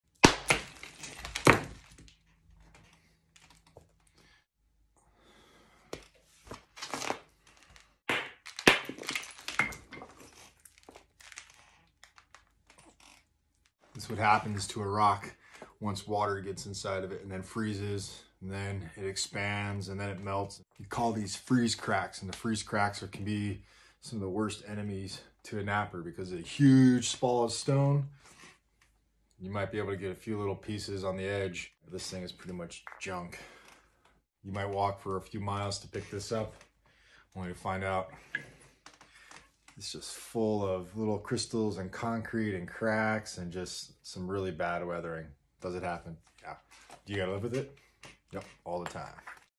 Junk Stone | Freeze Crakes and Weathered Stone flintknapping stone tools
This is a taste of what it like to open a piece of junk stone full of freeze crakes, crystals and cement pockets. Yes, a few flakes could be pulled from different edges to produce a small point, but overall its junk!

For full form videos check out @DonnyDustsPaleoTracks